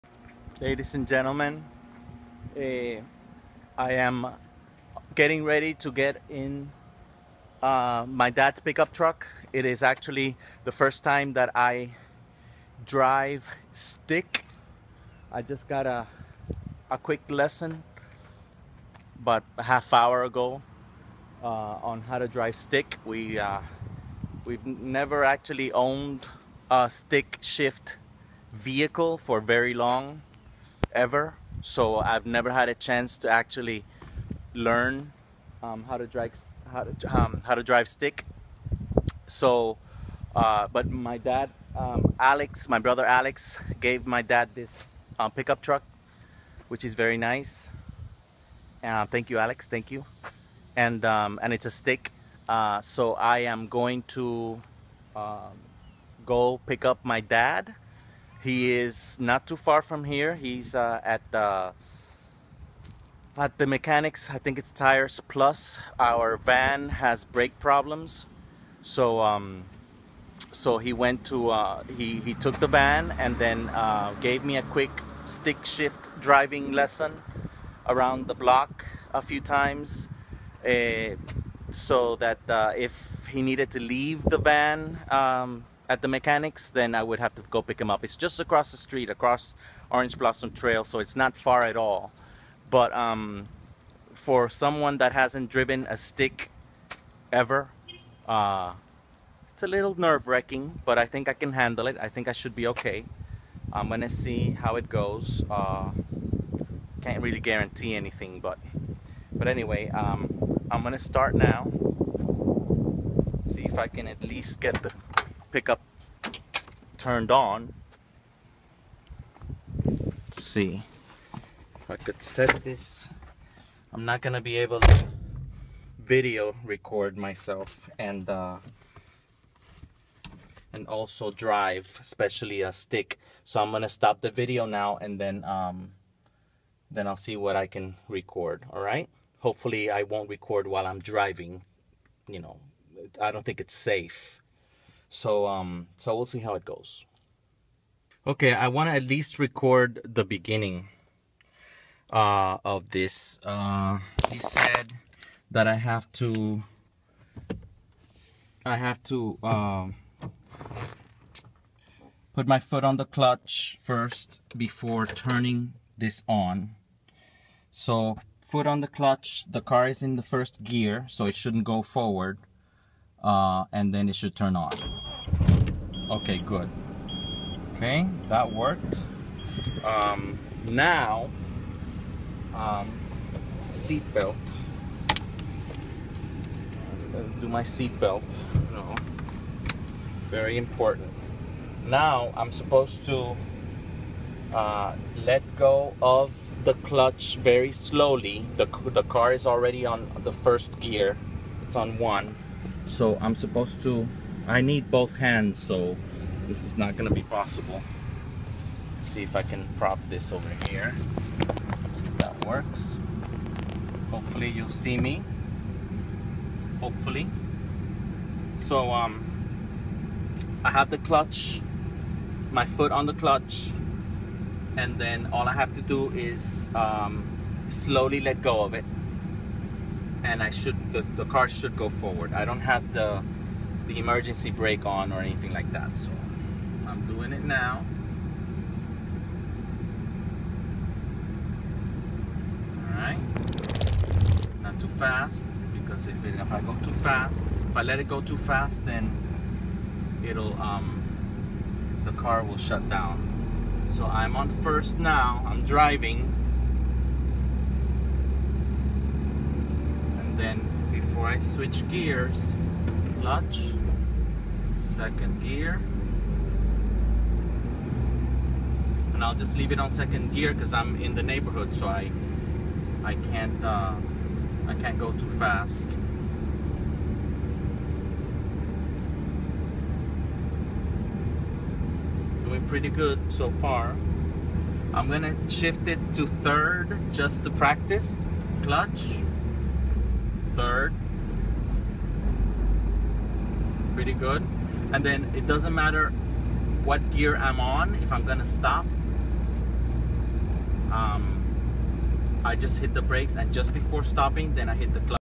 Driving Stick for the First Time! July 30, 2015
My dad gave me a quick 30 minute stick driving lesson about a half hour before I took this video! He dropped our van off at the shop to get new breaks, then called me to go pick him up. The break shop is just across the street on OBT, so it's basically just crossing the traffic light across. He gave me a quick lesson around the block a few times until I got the hang of it, sort of lol! I'm still very tense and not totally comfortable, but that will come with time and practice! July 30, 2015